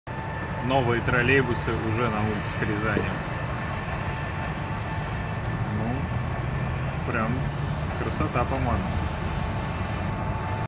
Новые тролейбусы на улицах Рязани
Тролейбусы Адмирал.
Новые, недавно закупленные, тролейбусы уже появились на улицах Рязани.
Я ещё лет 10 назад шутил, что у нас тралики сделаны из краски, так как железа под ней уже нет и всё держится на 15 слоях, разного цвета причём ))